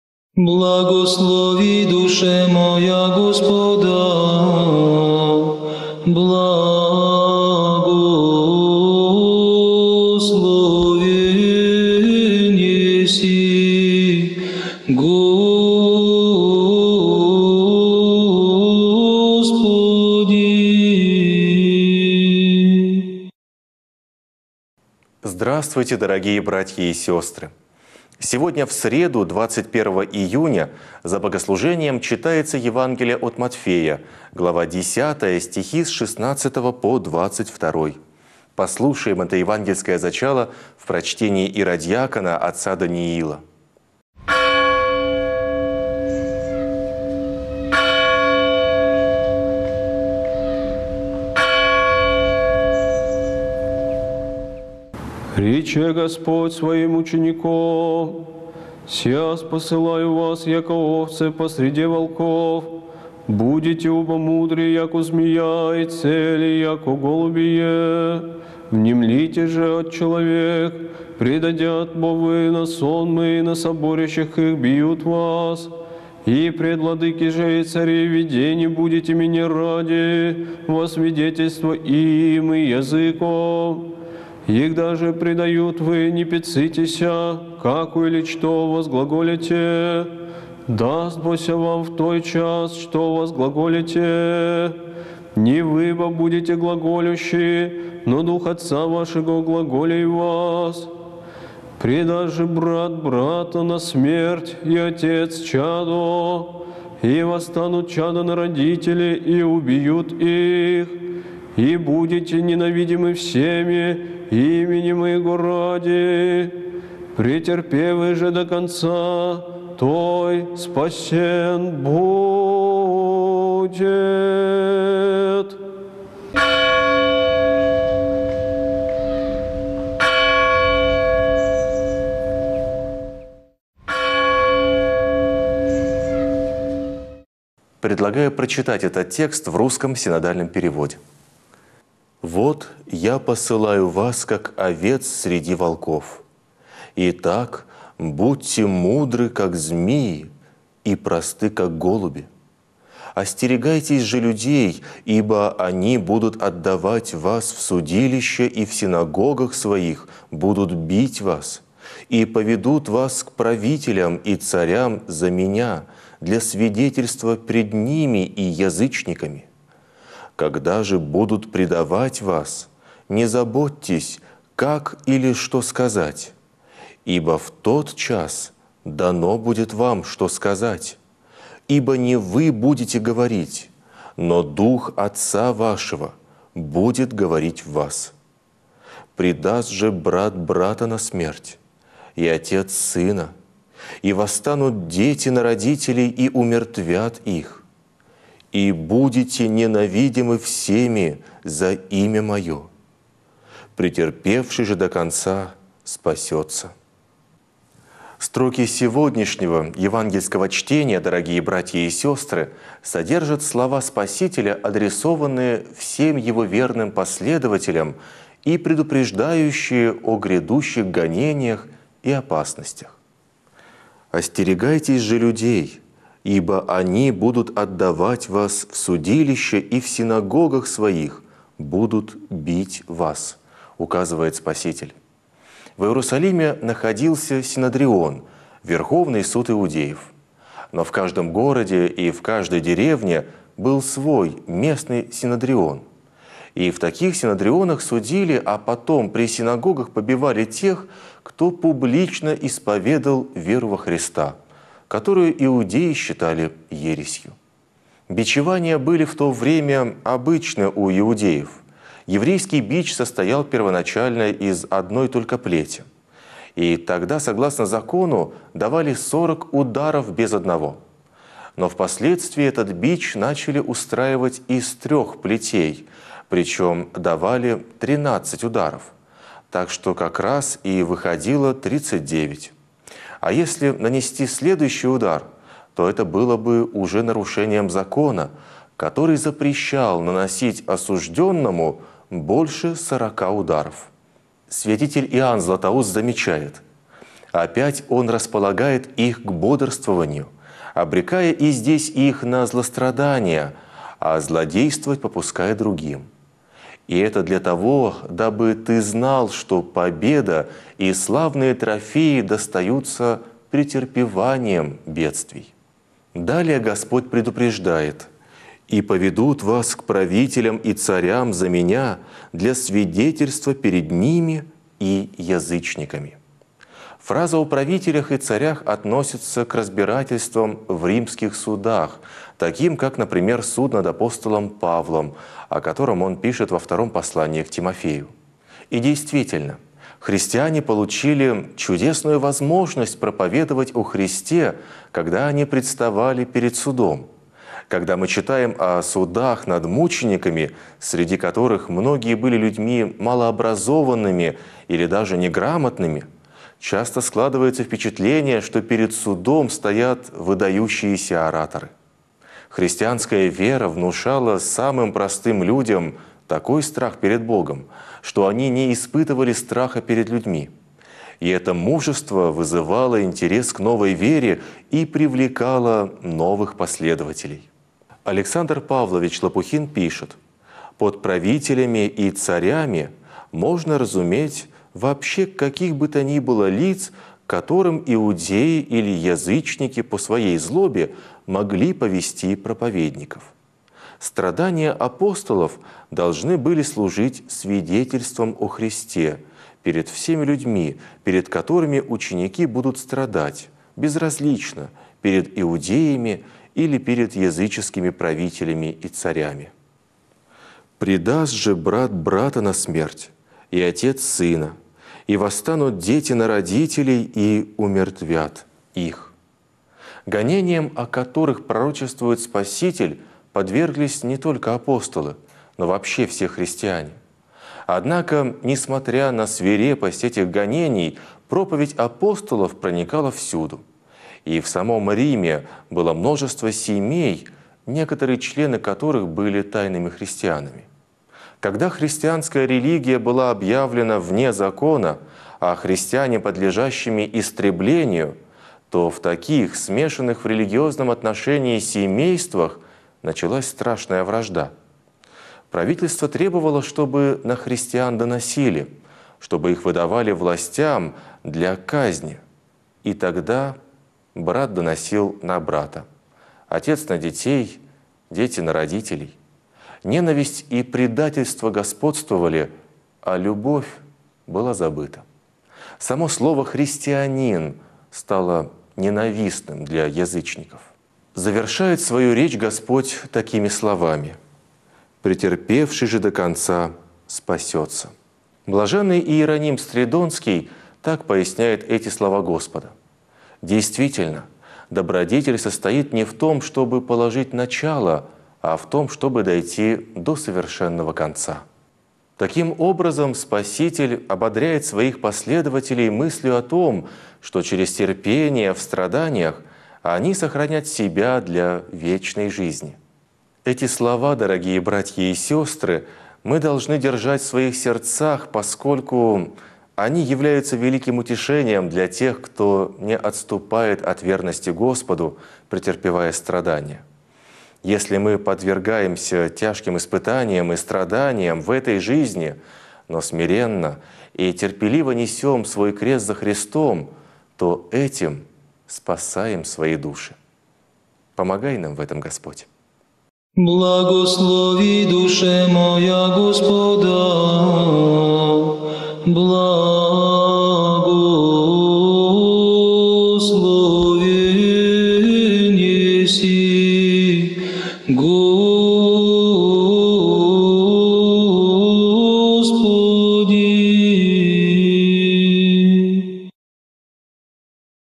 Читаем Евангелие вместе с Церковью 21 июня 2017г. Евангелие от Матфея. Глава 10, ст. 16–22.
Претерпевший же до конца спасется